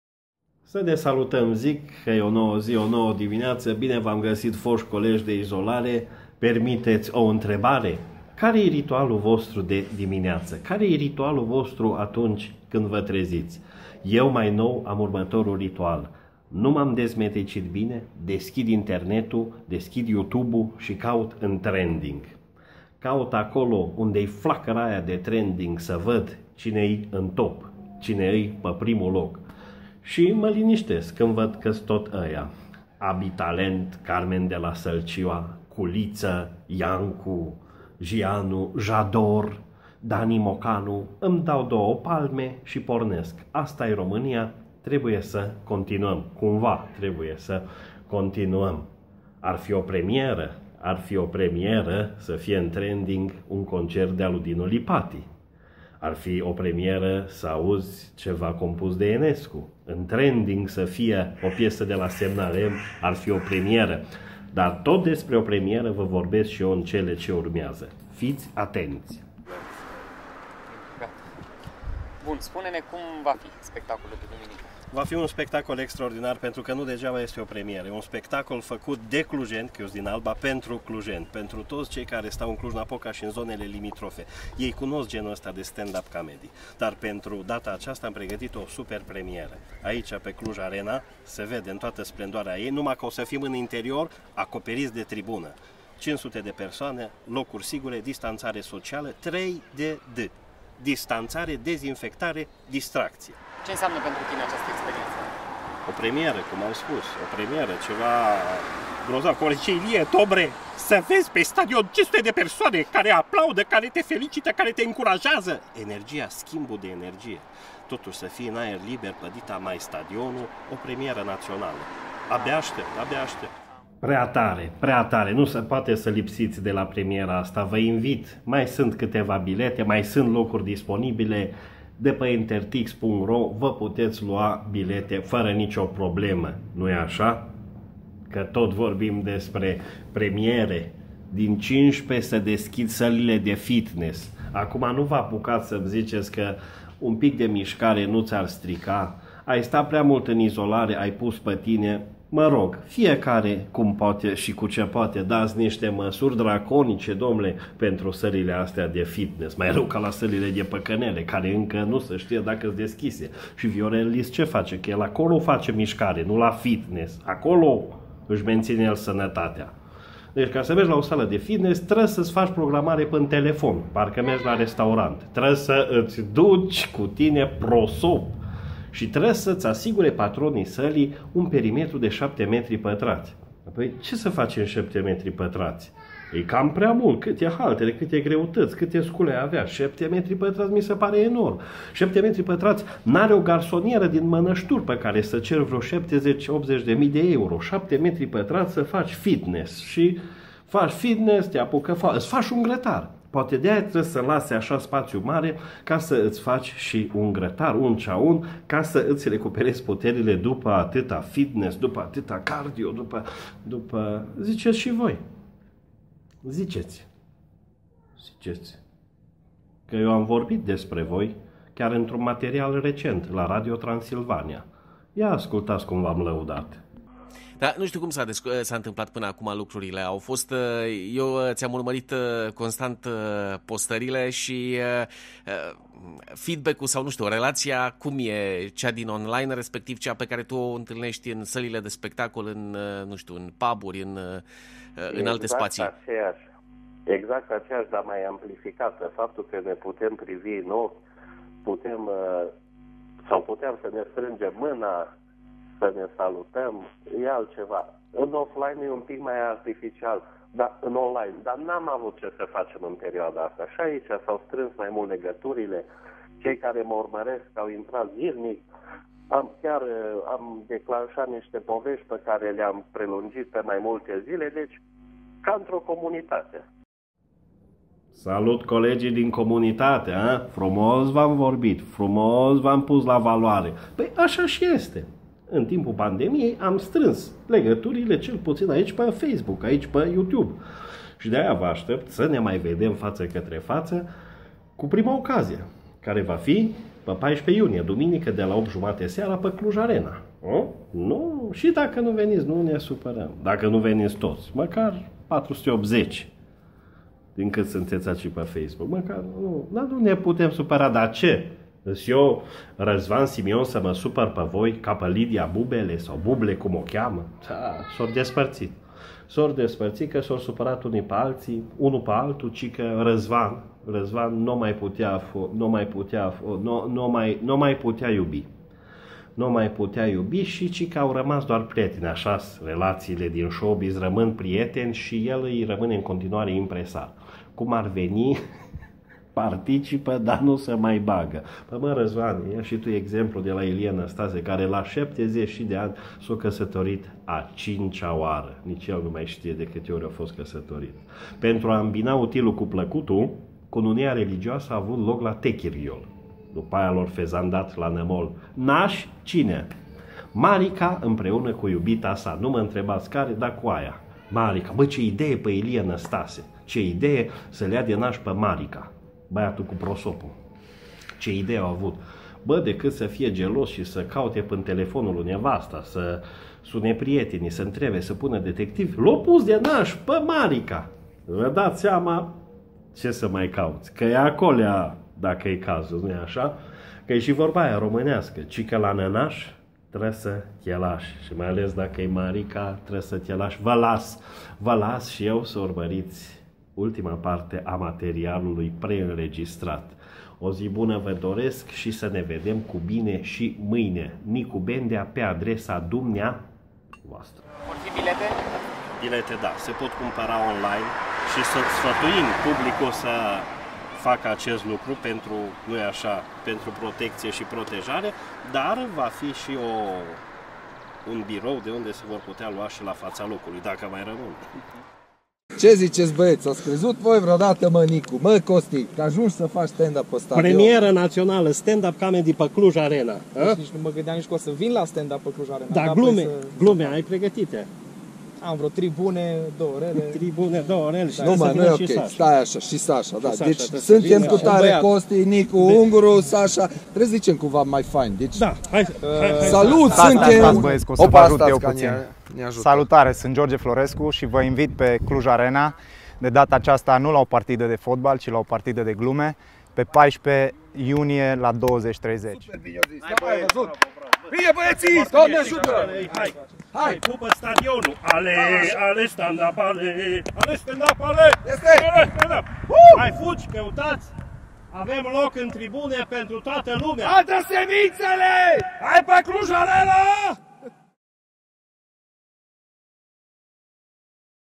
Nicu Bendea - Puțină mișcare pe Cluj Arena!
Stand up comedy pe Cluj Arena în premieră națională pe 14 iunie ora 20:30!